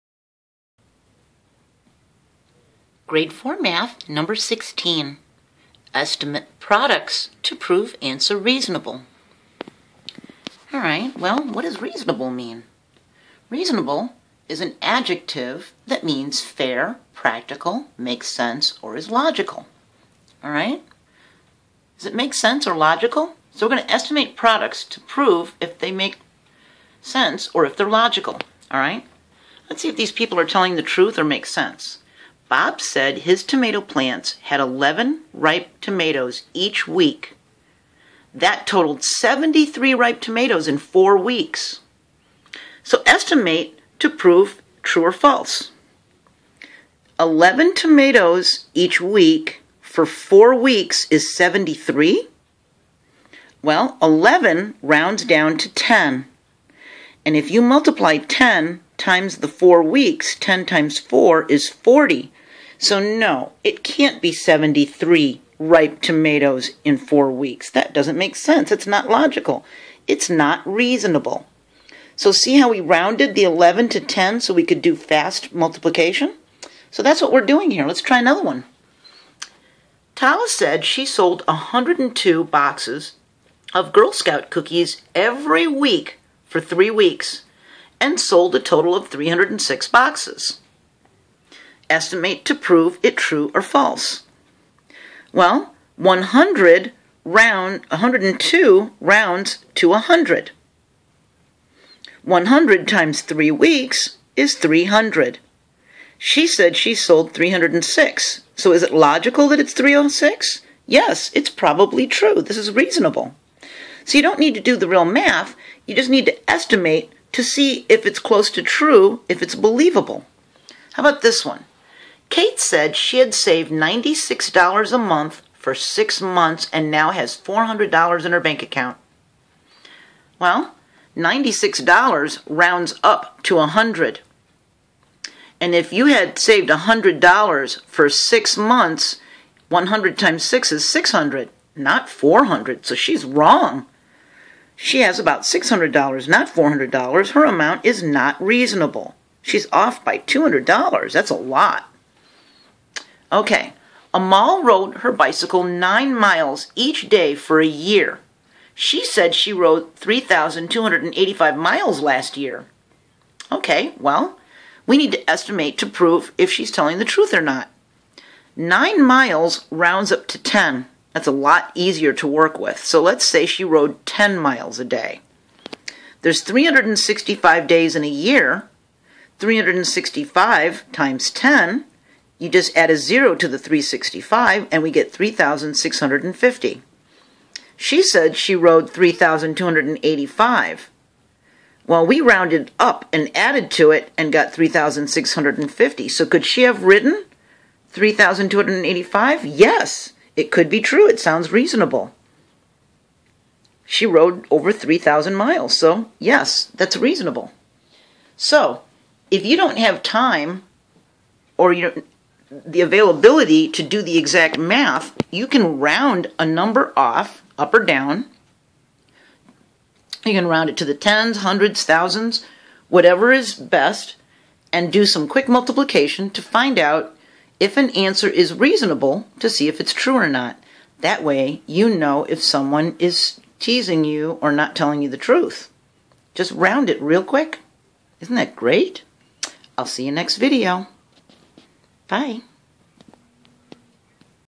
Grade 4 Math 2.4, Estimating Products to prove they're reasonable
How to estimate to prove an answer is reasonable or not. Estimating and rounding off  products to find if information is logical. #16